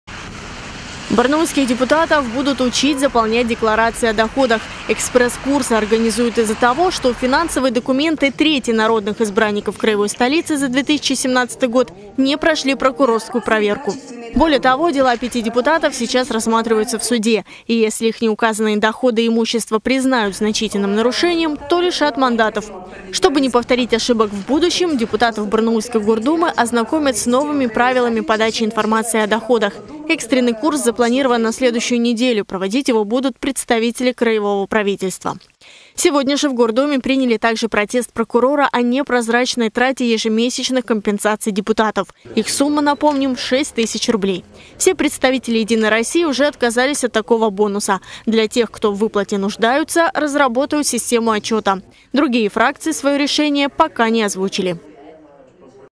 Барнаульских депутатов научат правильно заполнять декларации
Барнаульских депутатов будут учить заполнять декларации о доходах. Экспресс-курсы организуют из-за того, что финансовые документы трети народных избранников краевой столицы за 2017 год не прошли прокурорскую проверку.